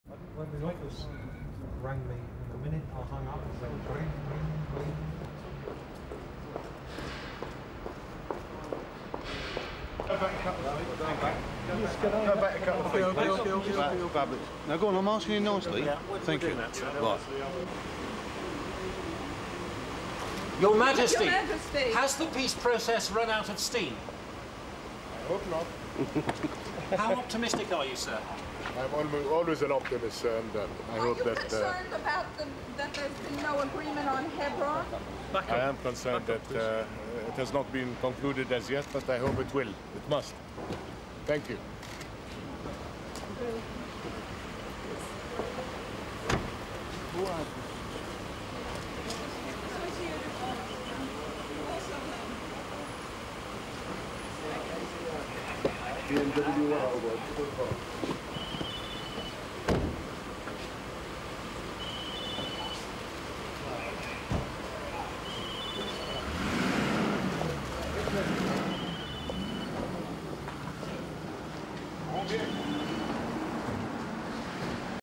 UK: LONDON: KING HUSSEIN OF JORDAN VISIT
King Hussein of Jordan, on a private visit to London, said Monday he was concerned at Israel's failure to conclude a deal with the Palestinians over the issue of Hebron.

But he added he remained optimistic about the Middle East peace process.

King Hussein addressed reporters after a meeting at the London offices of the Jewish Chronicle newspaper.

SOUNDBITE:
'Your Majesty, has the peace process run out of steam?'
'I hope not.'
'How optimistic are you, sir?'
'I'm always an optimist and I hope that, er...'
'Are you concerned that there's no agreement on Hebron?'
'I am concerned that it has not been concluded as yet, but I hope it will. It must.'

The King was then driven back to his hotel.